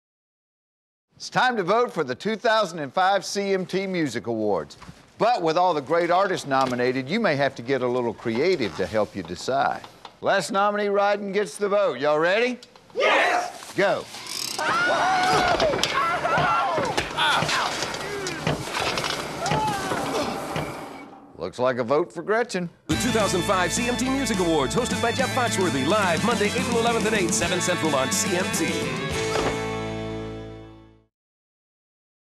CMT Music Award commercial "Jeff Foxworthy: Mechanical Bull"
(2005) One of three spots for the 2005 CMT Music Awards. Starring Jeff Foxworthy. Nice guy. Very funny and an absolute pro who was very easy to work with. Just a big, fun, dopey commercial high on sight gags and low on brains, but it was good time out in hotlanta (although it was cold as a bastard when we shot it).